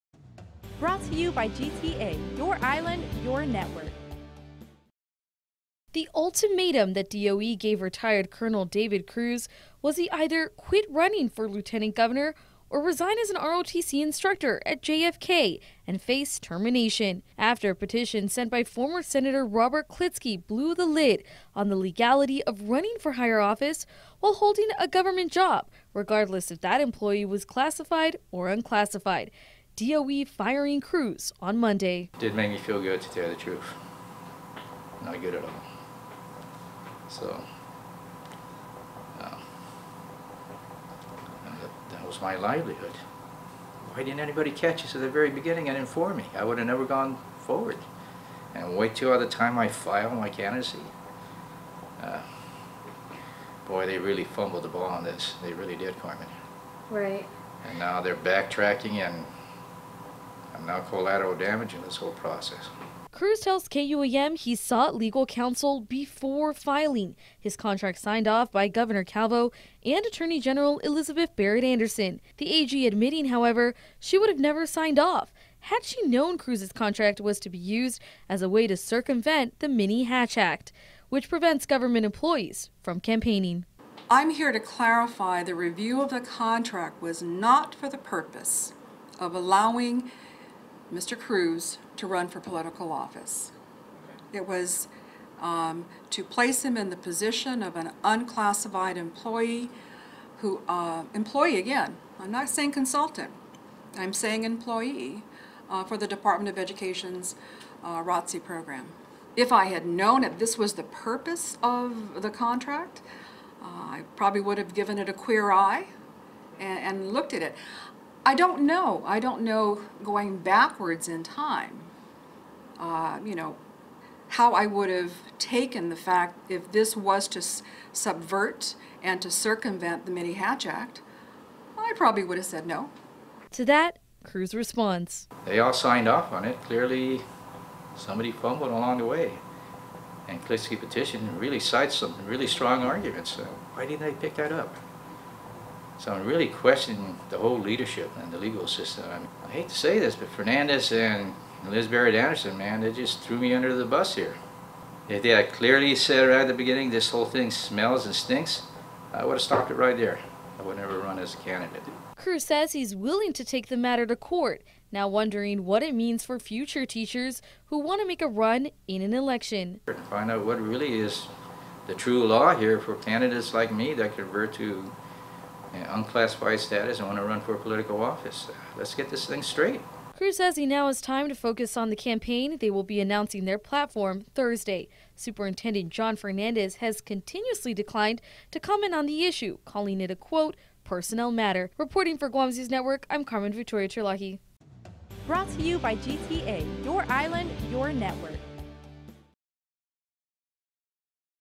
David Cruz says he may have reconsidered running if he knew he could be fired
Retired colonel and current candidate for lieutenant governor David Cruz is speaking out, admitting he might have reconsidered even running for public office if he knew firsthand the law that would ultimately cost him his job as a teacher.